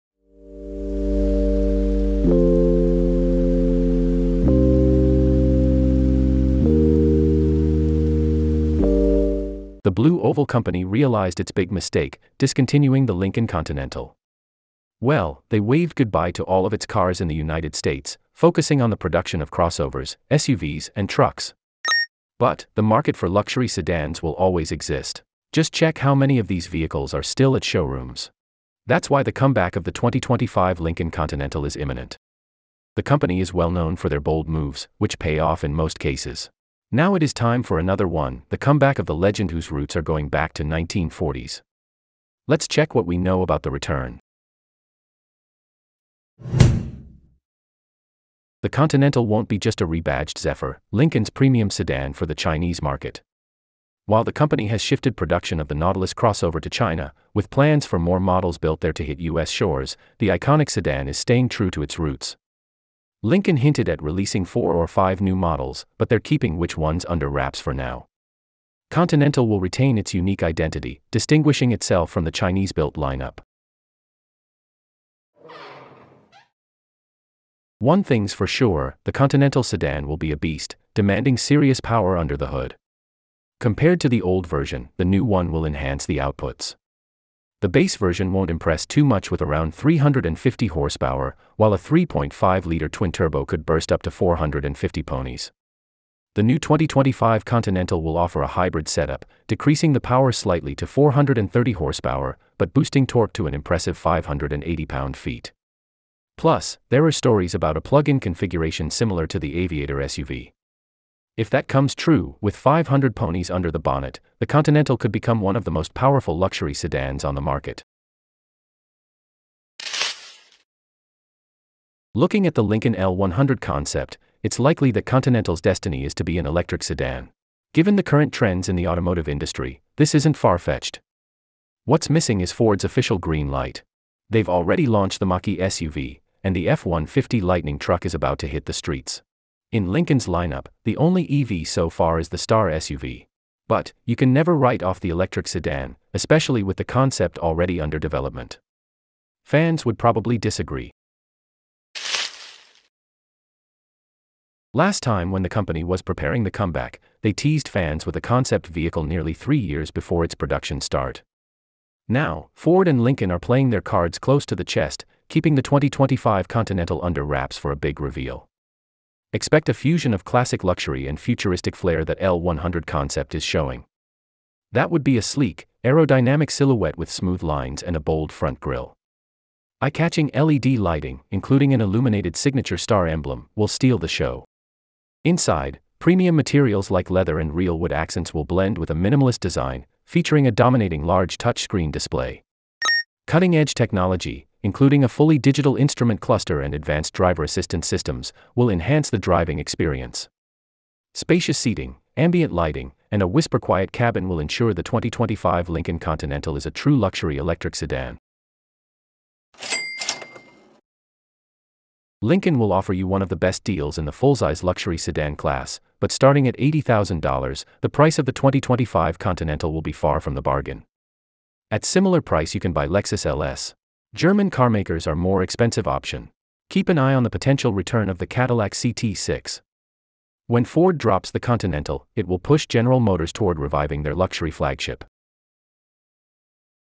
IT WAS A HUGE MISTAKE! 2025 Lincoln Continental is Coming Back
Big luxury sedans might not sell fast, but this is the safe market.

Giving up on Continental turned out to be a huge mistake and Ford is bringing it back. No, 2025 Lincoln Continental won't have anything to do with China - it won't be produced there and it won't be a rebadged Zephyr.

Supreme comfort, styling that combines elegant and sporty, potent engines - that's what the all-new premium sedan is going to offer.

Find out more about the 2025 Lincoln Continental comeback in our latest video.

Contents:
00:00 Intro
00:45 Will the new 2025 Continental be produced in China?
01:16 Specs and hybrid engine
02:00 Electric vehicle
02:31 Concept images
03:30 Price and competition